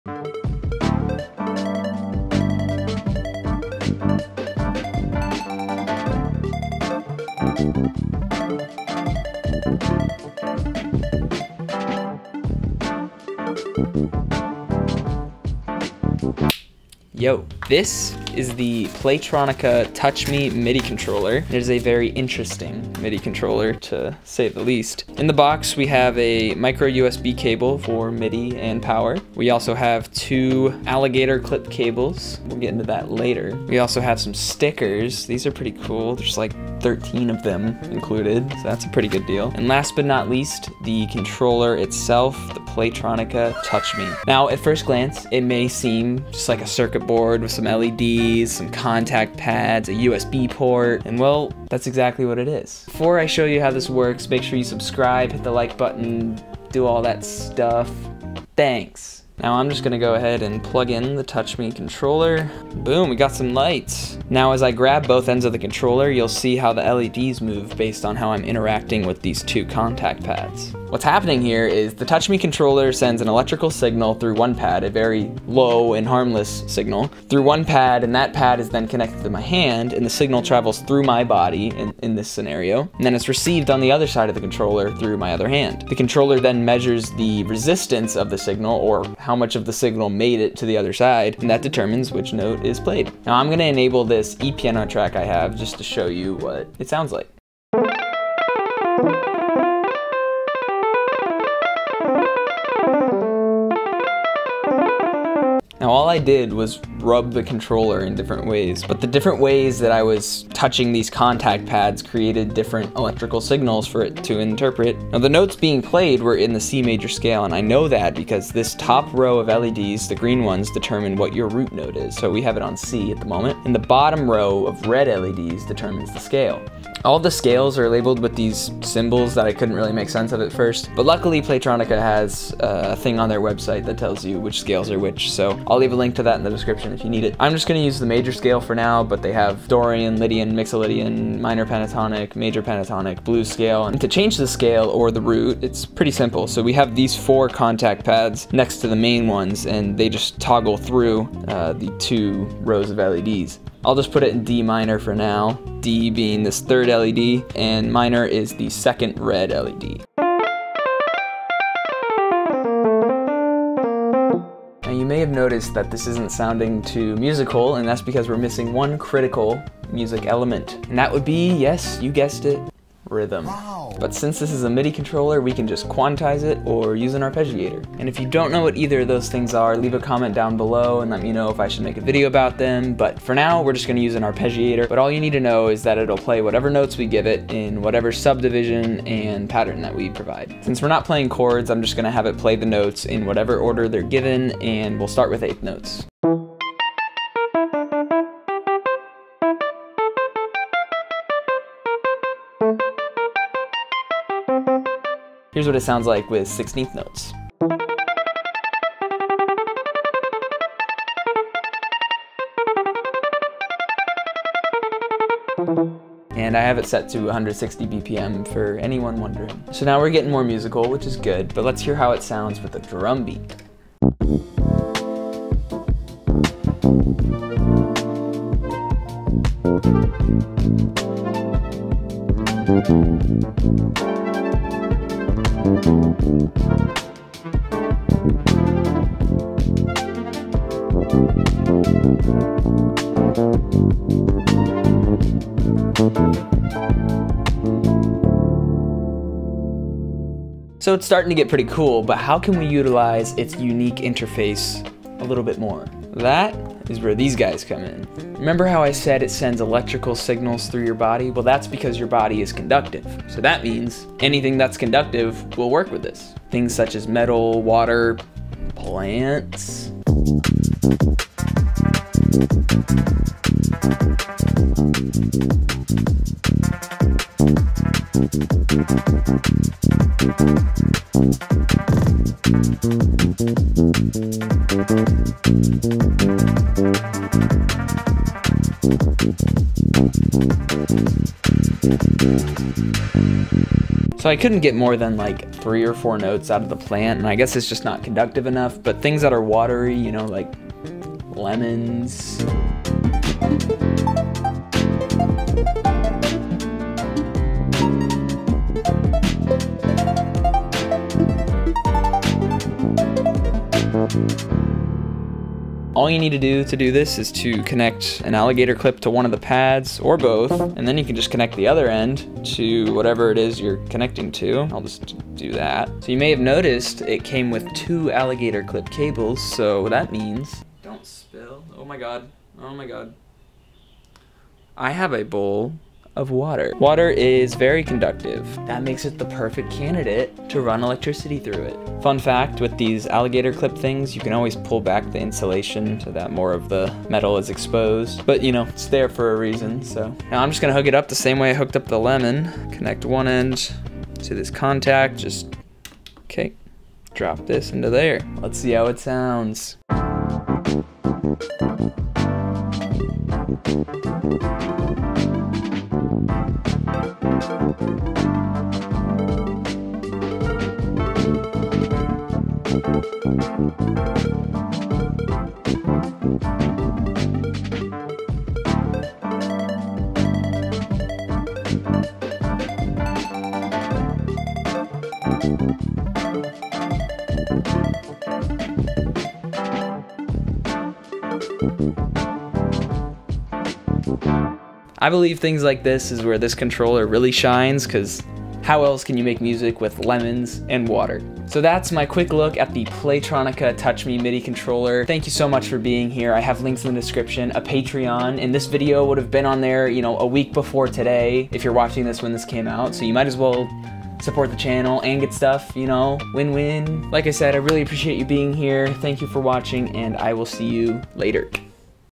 TouchMe - A Quick Look | Making Music with LEMONS
The TouchMe MIDI controller by @PlayTronica is one of the most unique controllers I've ever seen! I never thought that one day I would be making music with lemons and a bowl of water. This video is my first REAL YouTube video, so I really hope you enjoy it. I have lots of videos planned, and I plan to make more in the future!
Gil Tandeta Gregorio
Jim Lane
C-Dub
Chapters:
00:00 intro jam
00:17 unboxing
00:53 subscribe!
00:59 how the TouchMe works
01:37 what it sounds like
01:55 how to change key/scale
02:53 rhythm!
03:40 playing with a beat
04:11 utilizing the TouchMe interface
04:29 plant
05:01 lemon
05:31 adding water
06:42 outro